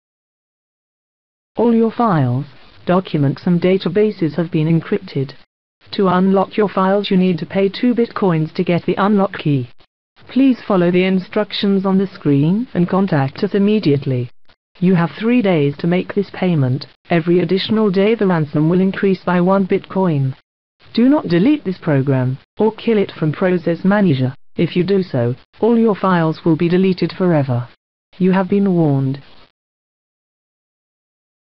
DetoxCrypto Ransomware Variant that takes a Screenshot of your Windows Screen
When this variant of the DetoxCrypto ransomware is installed, it will take a screenshot of the active Windows screen and upload it to the developer.